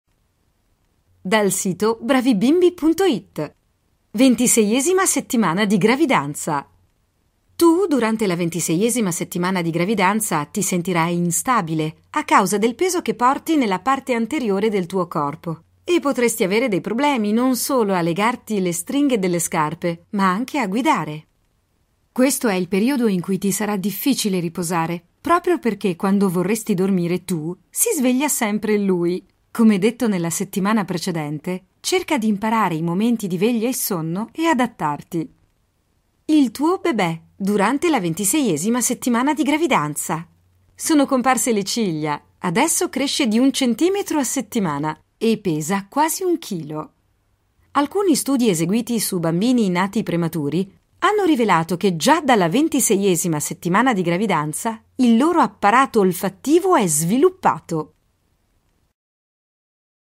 ventiseiesima settimana gravidanza
Guida alla ventiseiesima settimana di gravidanza.
Arrivata alla ventiseiesima settimana (26) di gravidanza ti sentirai instabile a causa del peso che porti nella parte anteriore del tuo corpo!
Ascolta l'audio guida alla gravidanza.